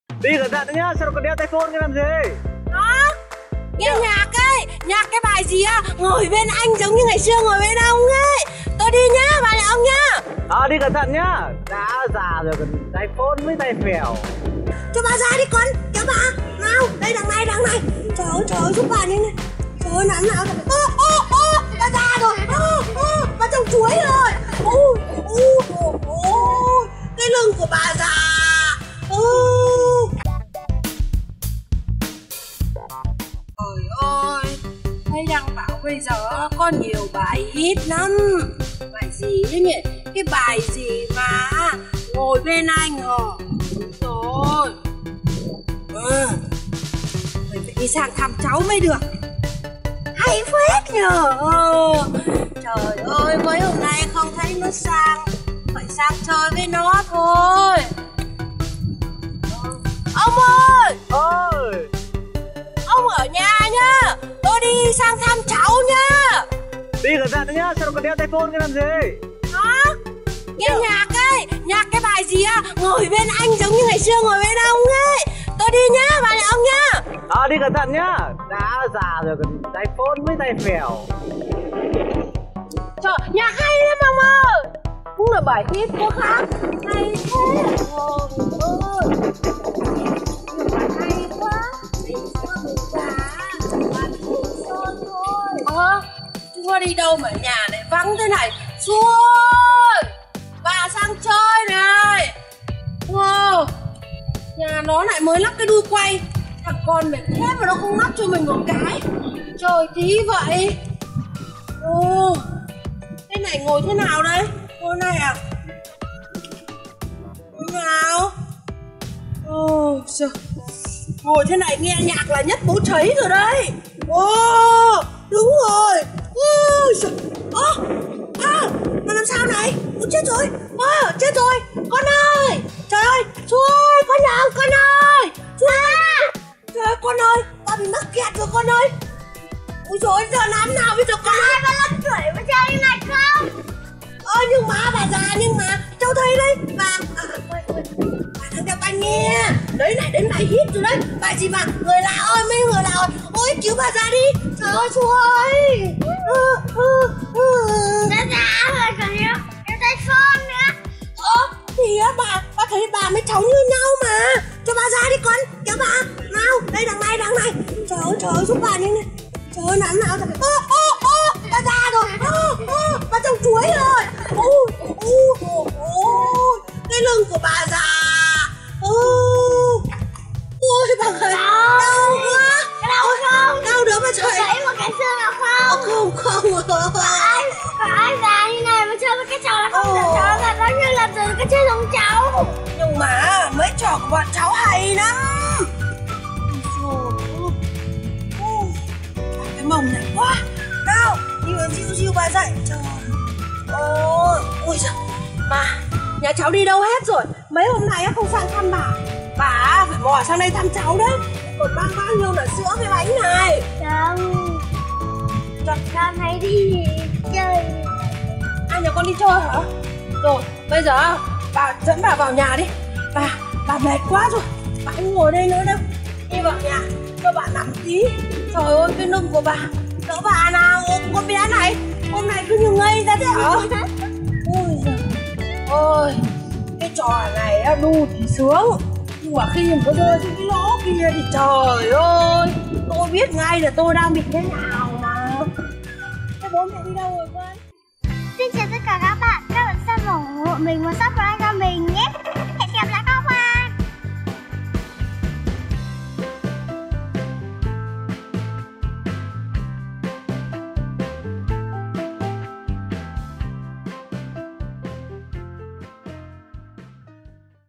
Bà Già Xì Tin Ngồi Xích Đu ❤Susi kids TV❤
Bà Tuy gia nhưng rất là xì tin hôm nay sang thăng nhà Susi nhưng vớ ngay được cái xích đu thế nào ngồi thế nào bị chui tọt xuống dưới không thể nào ra được may có cô cháu bé nhỏ ra đỡ bà chứ không thì hihi.